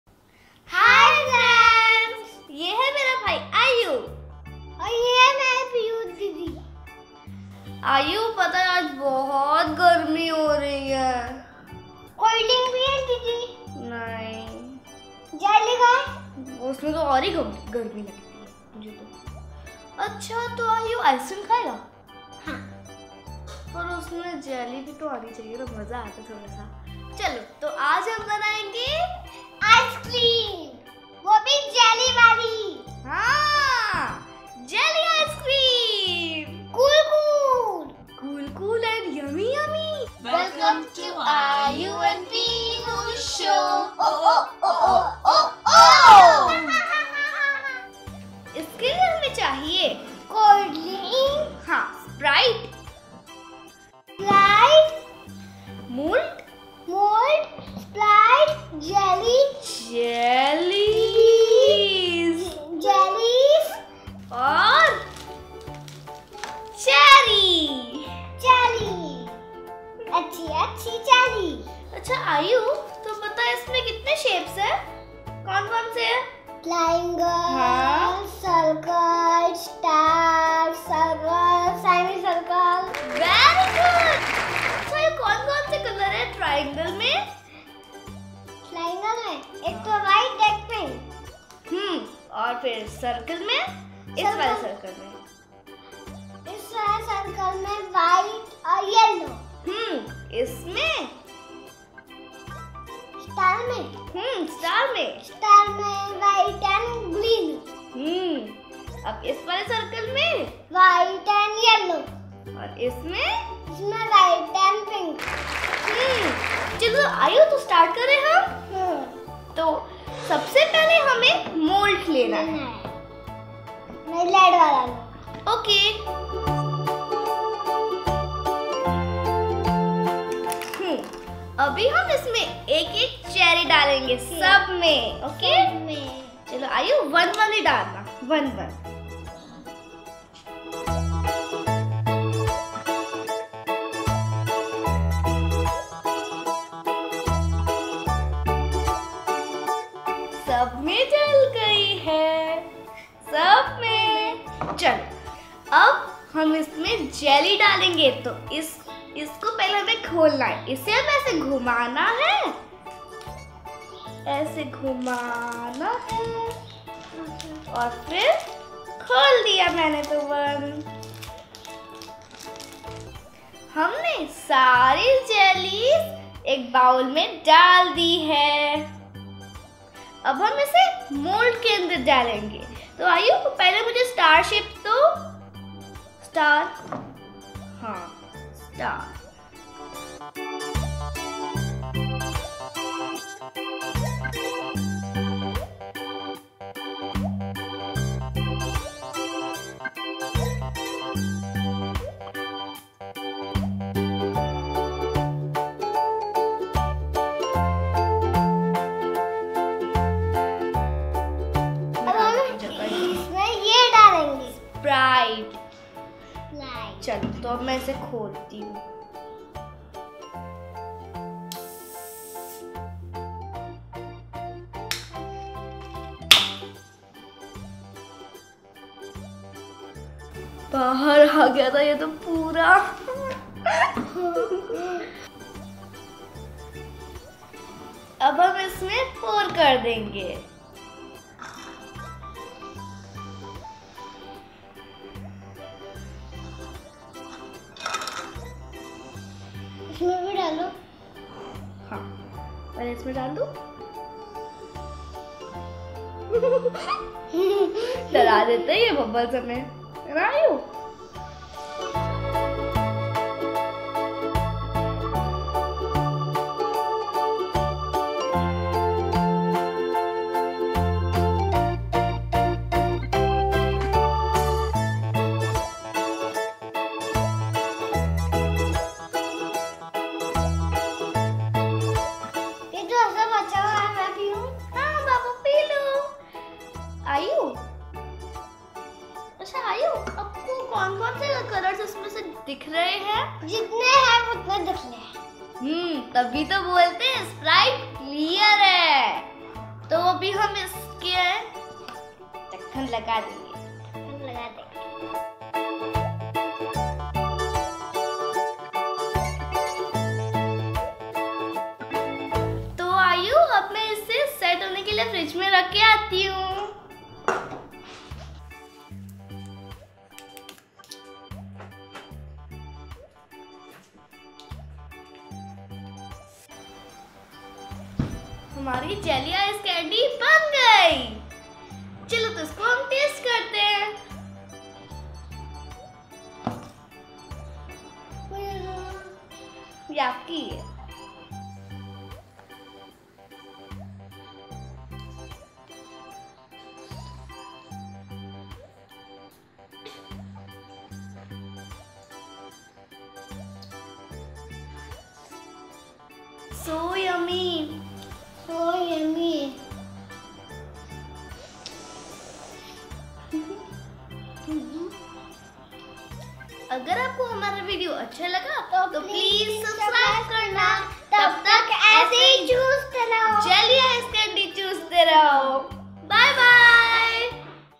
JELLY ICE CANDY | Learn colours shapes | Kids Cooking real food | Aayu And Pihu Show
Learn how to make Jelly Ice Candy -
Sprite (You can use any cold drink or juice)
Colorful Jelly, Ice cream mold
Parent-friendly, happy family show - If you like our videos, please share.

Copyright @ Aayu and Pihu Show.